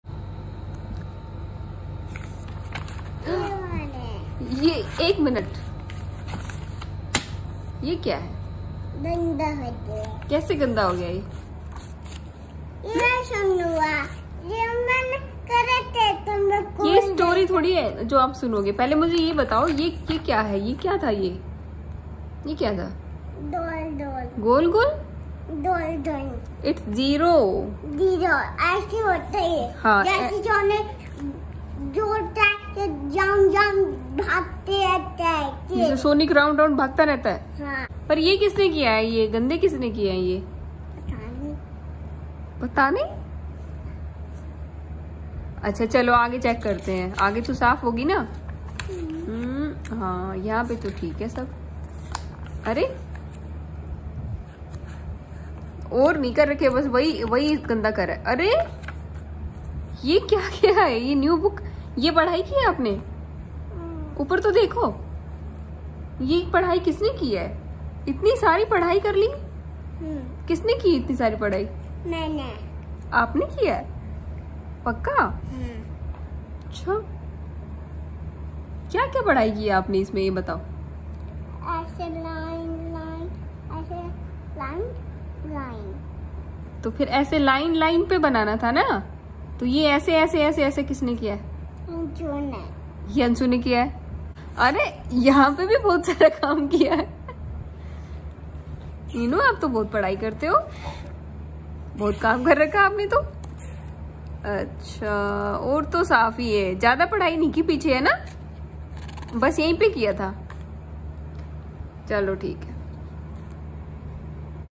Devina ki funny stories about study आज देखो कैसी पढ़ायी करती है और क्या बातें बनाती है 🤣
Devina ki funny stories about study
Bahut pyari batein banati h